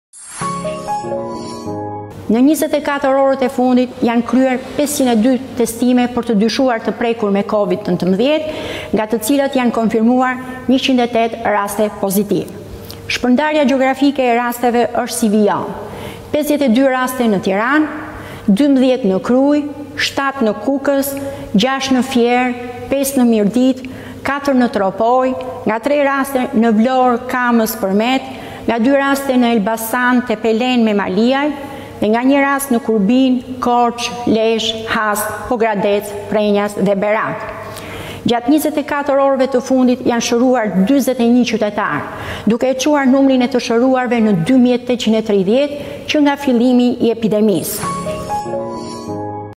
108 persona të infektuar me koronavirus sot, qytetet ku u konfirmuan rastet e reja
Në vendin tonë janë regjistruar 108 raste të reja me koronavirus në 24 orët e fundit nga 502 testime të kryera.

Mirela Cami, drejtore e Operatorit të Shërbimeve të Kujdesit Shëndetësor bëri të ditur se numri total i të infektuarve me koronavirus në vendin tonë ka arritur në 5105.

Qytetet ku janë konfirmuar rastet e reja janë Tirana me 52 raste, 12 në Krujë, 7 në Kukës, 6 në Fier, 5 në Mirditë, 4 në Tropojë, nga 3 raste në Vlorë, Kamëz, Përmet, nga 2 raste në Elbasan, Tepelenë, Memaliaj, nga 1 rast në Kurbin, Korçë, Lezhë, Has, Berat, Pogradec, Prrenjas.

Dy ishin viktimat sot nga COVID-19, duke e çuar numrin e të vdekurve në 150. Aktualisht në spitalin Infektiv dhe “Shefqet Ndroqi” por marrin trajtim 125 pacientë. 17 prej tyre në terapi intensive ndërsa 7 të intubuar. 41 pacientë janë shëruar në 24 orët e fundit.

Bilanci

2 të vdekur
Fatkeqësisht, nuk e kanë fituar dot betejën me sëmundjen në spitalin Covid1, një 68 vjeçare nga Shijaku me disa sëmundje bashkëshoqëruese. Ka humbur jetën në spitalin Covid2 “Shefqet Ndroqi, një 60 vjeçare nga Tirana, pa sëmundje të tjera.

125 pacientë në spitale
Aktualisht në spitalin Infektiv dhe spitalin “Shefqet Ndroqi” po trajtohen 125 pacientë, 17 janë në terapi intensive dhe 7 pacientë janë të intubuar. Në 24 orët e fundit, janë kryer 502 testime për të dyshuar të prekur me COVID19, nga të cilat janë konfirmuar 108 raste pozitive.

Rastet e reja sipas qyteteve
52 raste në Tiranë, 12 në Krujë, 7 në Kukës, 6 në Fier, 5 në Mirditë, 4 në Tropojë, nga 3 raste në Vlorë, Kamëz, Përmet, nga 2 raste në Elbasan, Tepelenë, Memaliaj, nga 1 rast në Kurbin, Korçë, Lezhë, Has, Berat, Pogradec, Prrenjas.

41 të shëruar
Gjatë 24 orëve të fundit janë shëruar 41 qytetarë, duke e çuar numrin e të shëruarve në 2830, që nga fillimi i epidemisë.

COVID-19 – Statistika (29 Korrik 2020)
Testime totale – 40192
Raste pozitive 5105
Raste të shëruara 2830
Raste Aktive 2125
Humbje jete 150 (Qarku Tiranë 80, Shkodër 23, Durrës 20, Fier 6, Elbasan 5, Kukës 4, Vlorë 4, Korçë 3, Dibër 2, Lezhë 2, Berat 1)

Shpërndarja gjeografike e rasteve aktive sipas qarqeve:

Tiranë 1146
Durrës 255
Shkodër 202
Vlorë 105
Lezhë 83
Fier 76
Korçë 69
Kukës 60
Elbasan 49
Berat 30
Gjirokastër 29
Dibër 21

Apeli për qytetarët
Ministria e Shëndetësisë dhe Mbrojtjes Sociale i bën thirrje edhe njëherë bizneseve, bareve, restoranteve institucioneve, të zbatojnë me korrektësi protokollet e sigurisë në punë, të miratuara nga autoritetet shëndetësore. Telefononi në numrin unik të Urgjencës Kombëtare 127 nëse keni shenja të sëmundjes, dhe në linjën e gjelbër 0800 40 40 për informacion rreth Covid19 apo këshillim psikologjik.